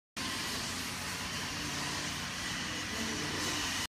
Animatronic Monkey
Video of the animatronic monkey, production hasn't completed yet.